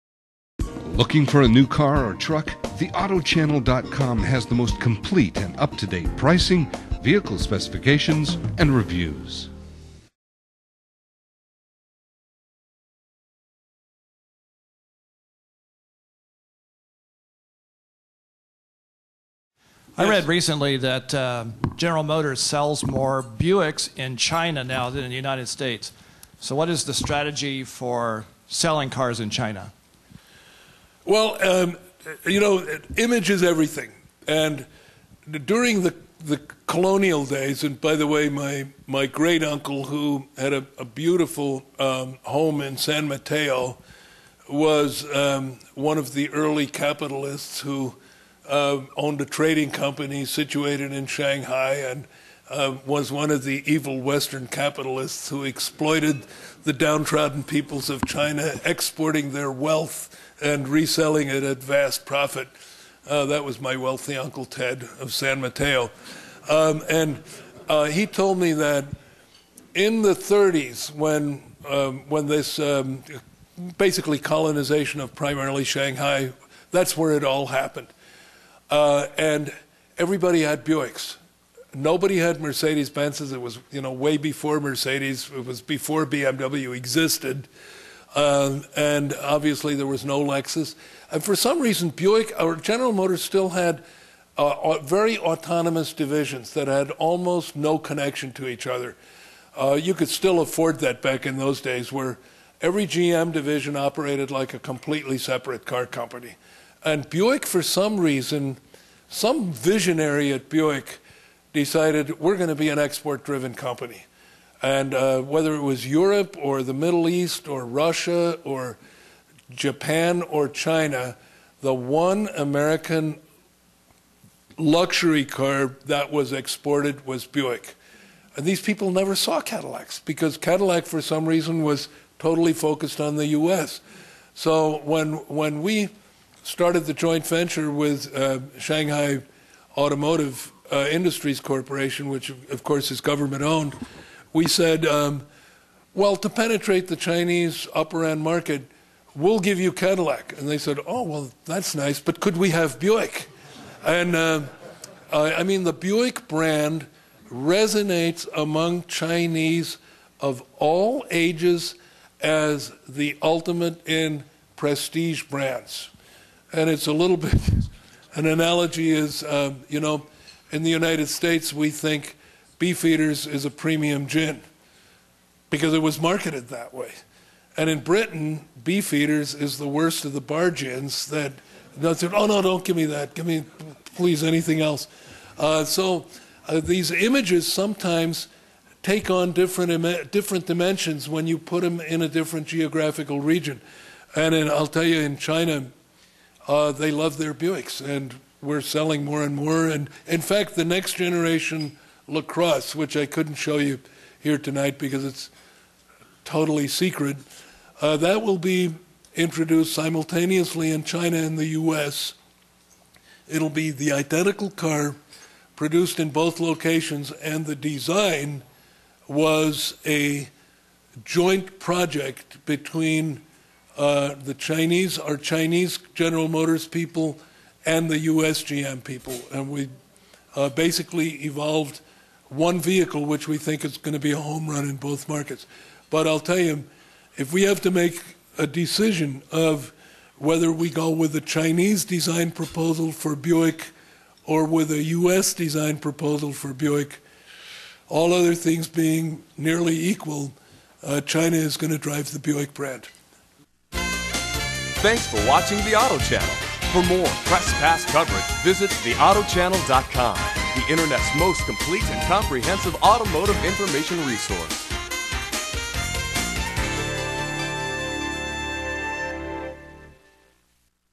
Bob Lutz @ WAJ 2007 - Q&A pt. 1 - China Strategy
In November 2007, Bob Lutz delivered a long presentation at the Western Automotive Journalists meeting in Northern California. The presentation is notable because of the multitude of topics that Bob touched on, and because it was the year before all of GM's financial woes came to light.